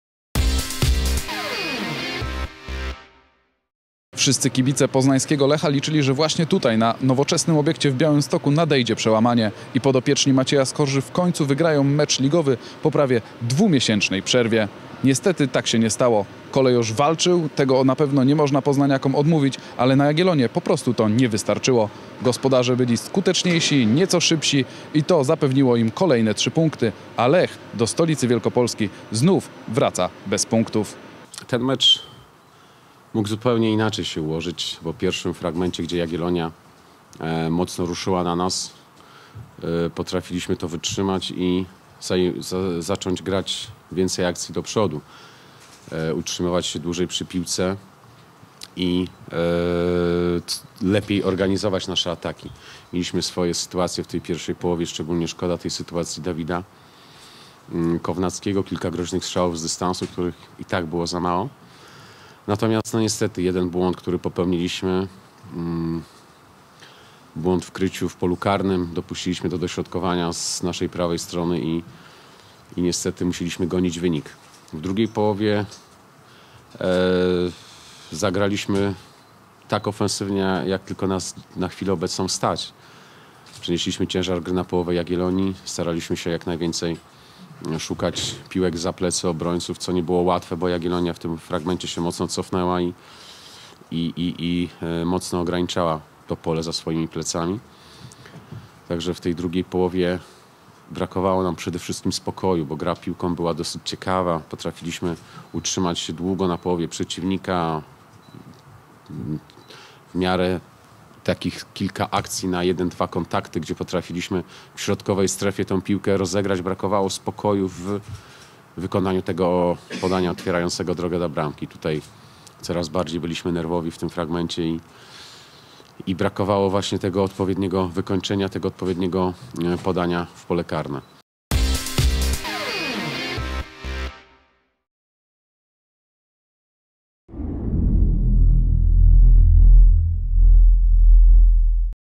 Lech przegrywa po raz siódmy
W 9. kolejce Ekstraklasy Lech Poznań doznał siódmej porażki. Tym razem Kolejorz przegrał 0:1 na wyjeździe z Jagiellonią Białystok.